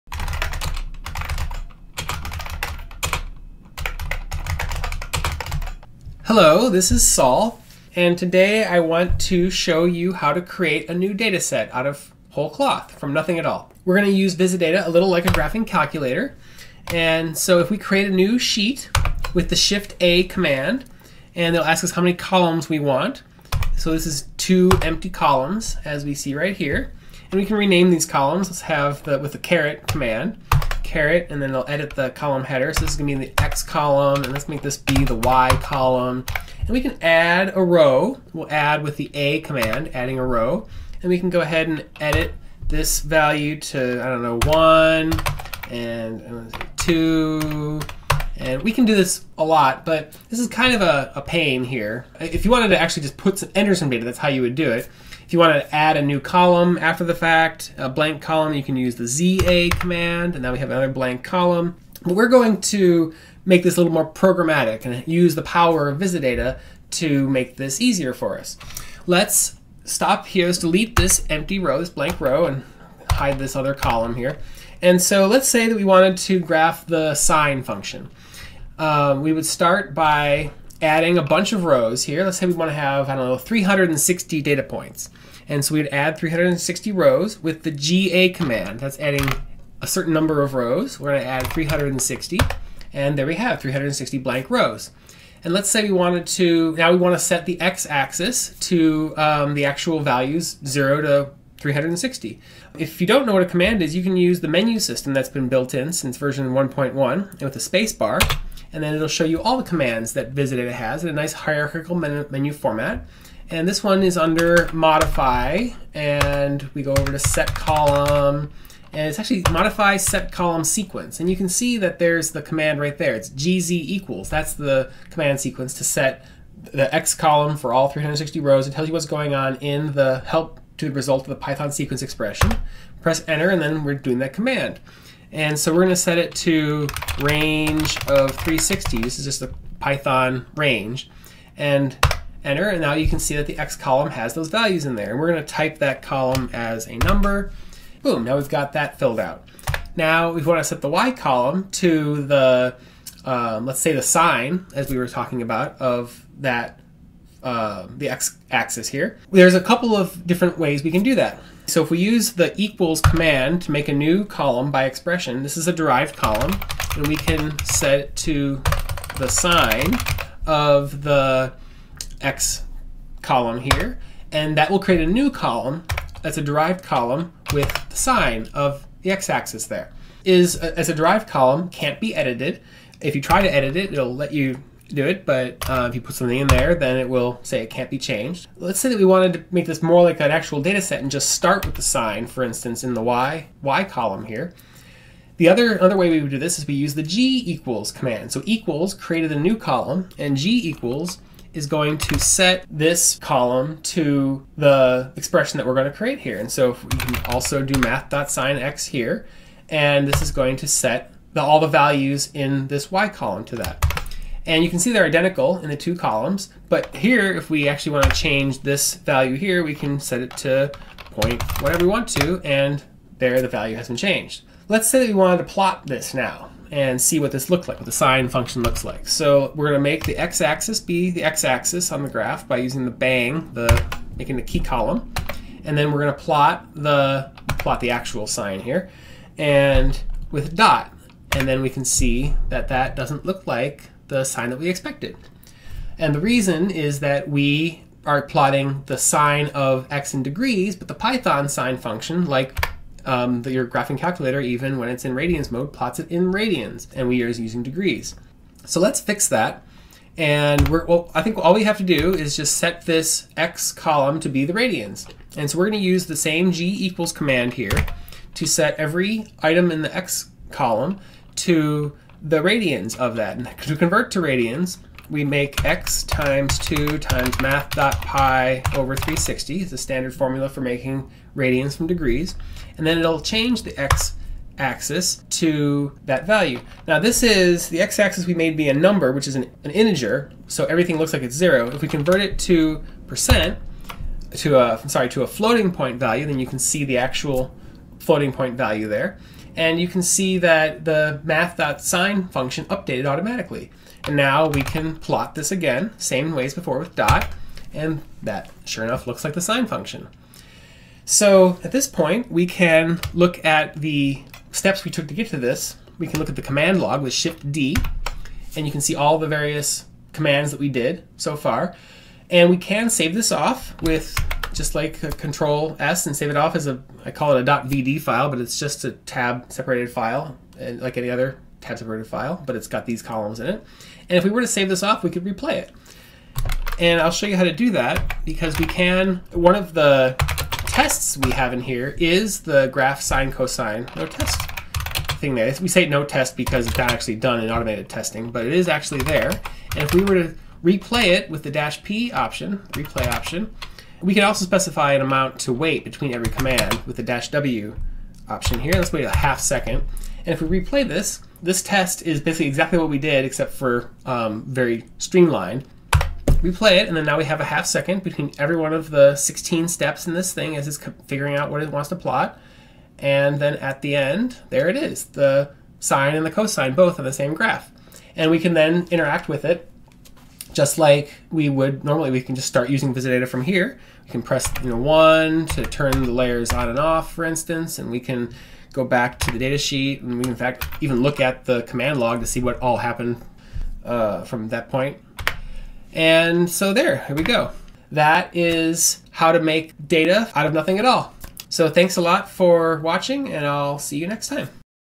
VisiData Case Study #3: graphing calculator
Using VisiData v1.1 to create a dataset on a blank sheet, graph it, and then replay a .vd file.

Commands used:

★ 0:17 Shift-A to create a new sheet
★ 0:27 ^ (caret) to rename a column
★ 0:38 'a' to add a blank row
★ 0:41 'e' to edit
★ 0:55 'za' to add an empty column
★ 1:26 'ga' to add N rows
★ 1:43 SPACE to browse the menu of all commands
★ 2:00 gz= to set column cells to Python sequence (modify-set-column-sequence)
★ 2:42 = (equals) to make a derived column from Python expression (not-modifiable)
★ 3:15 g= to set column values to Python expression (modifiable)
★ 3:50 ! (bang) to set key column (x-axis on plot)
★ 3:55 . (dot) to plot numeric column
★ 4:25 g= to modify column values
★ 4:50 % (percent) to set column type to float
★ 5:16 Shift-D to open the cmdlog (Ctrl-S to save the .vd)
★ 5:55 replay with -w (wait)